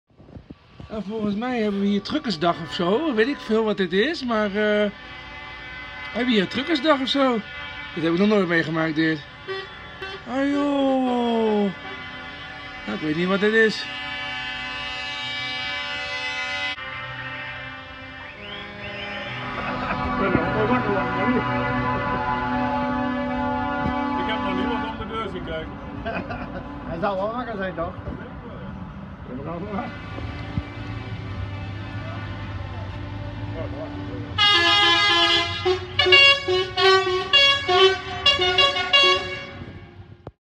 Vrachtwagen optocht bij wilblues (tv)
Dit heeft ook nog steeds met de verjaardag van de bewoner te maken